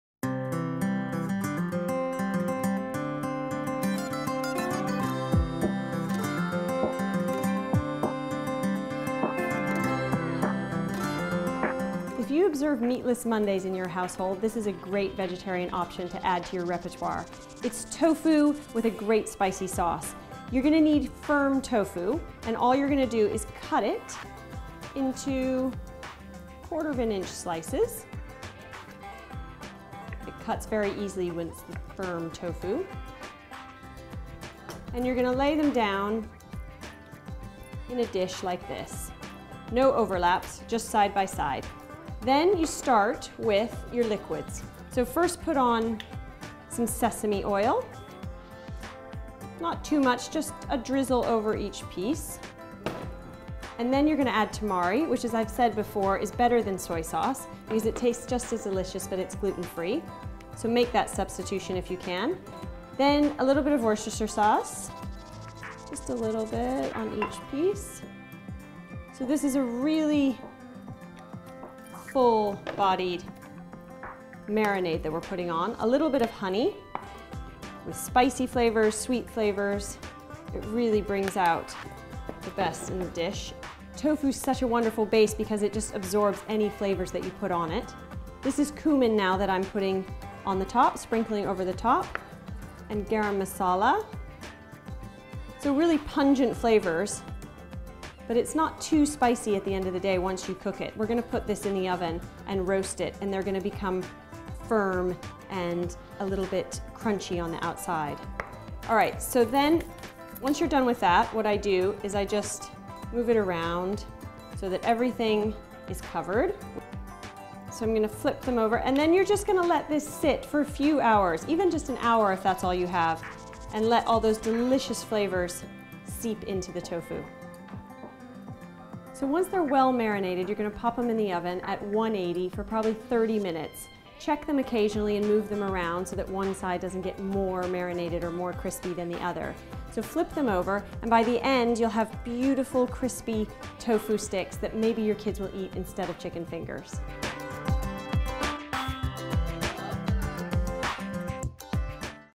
Tasty Tofu "Steaks"
This dish is great for Meatless Mondays or whenever you're looking for an alternative to chicken fingers for your kids. It's bursting with vibrant flavours and hardly takes any time at all to prepare.